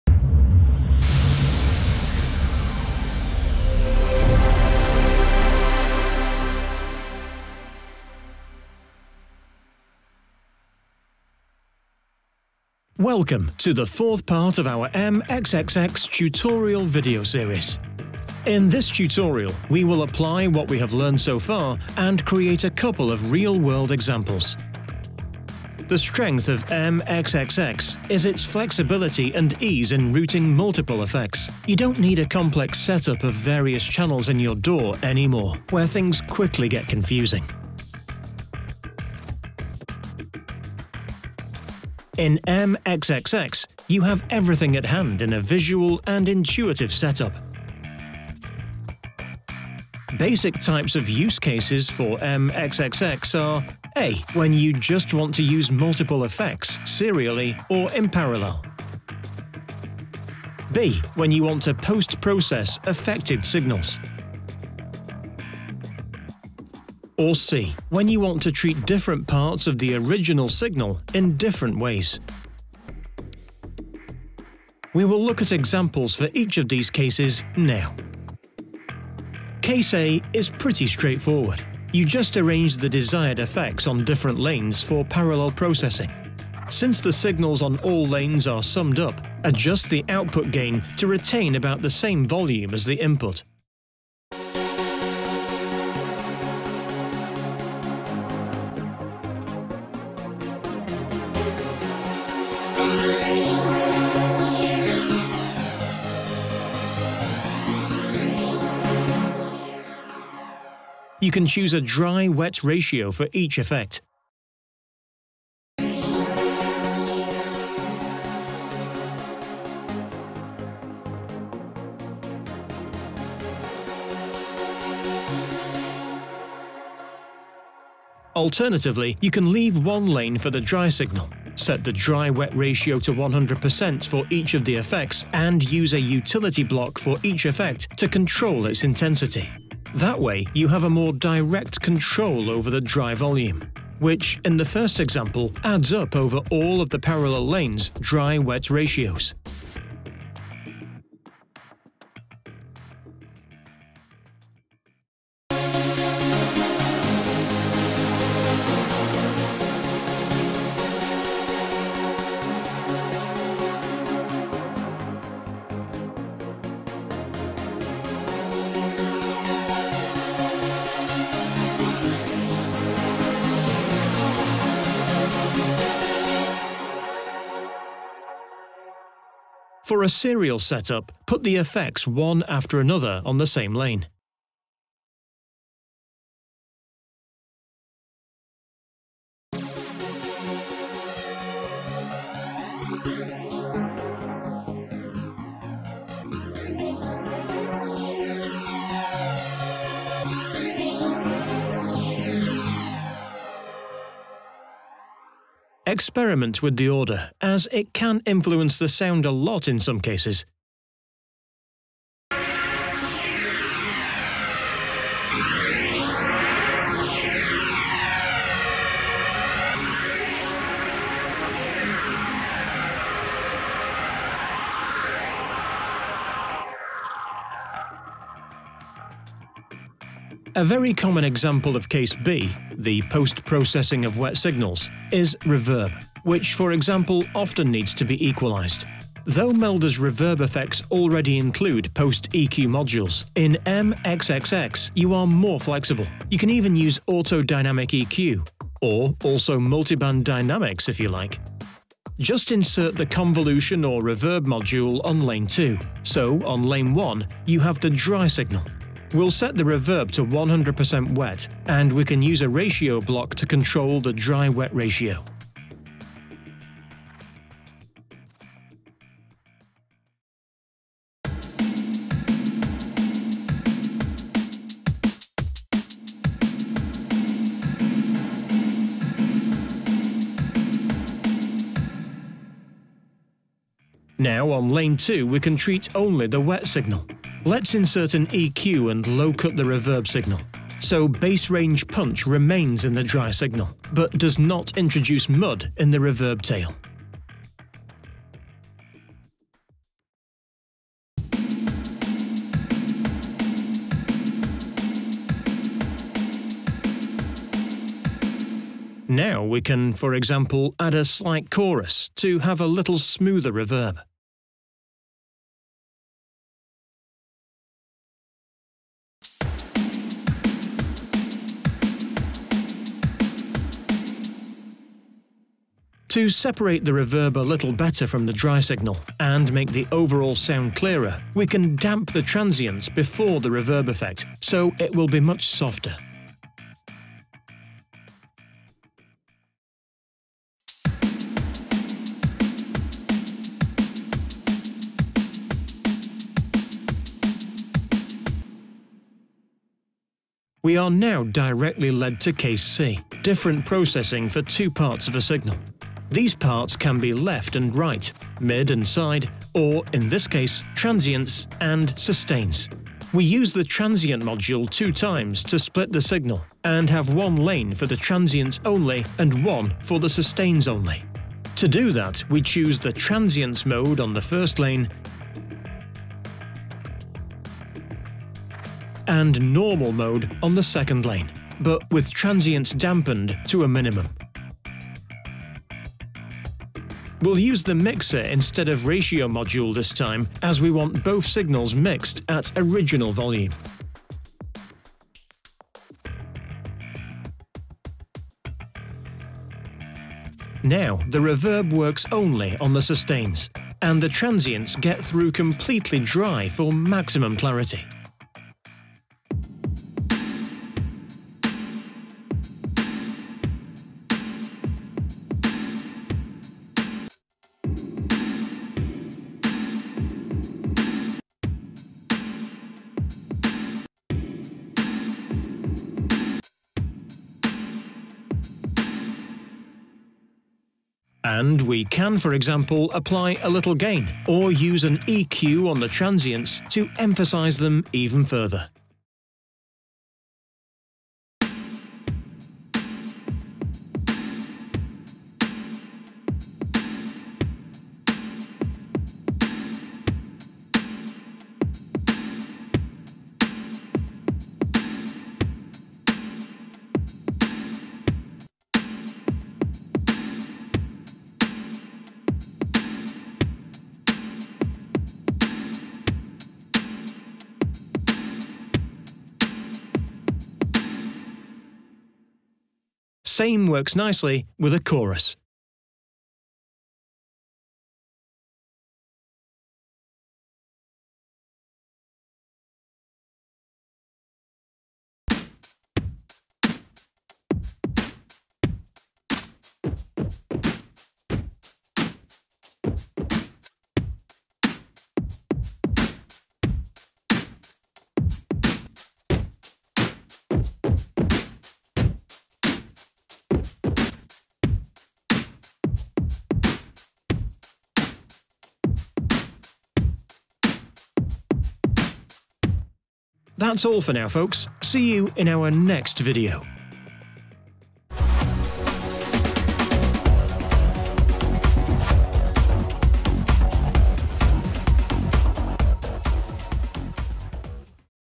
MXXX examples #1
The 1st MXXX example tutorial shows some basic serial/parallel processing ideas.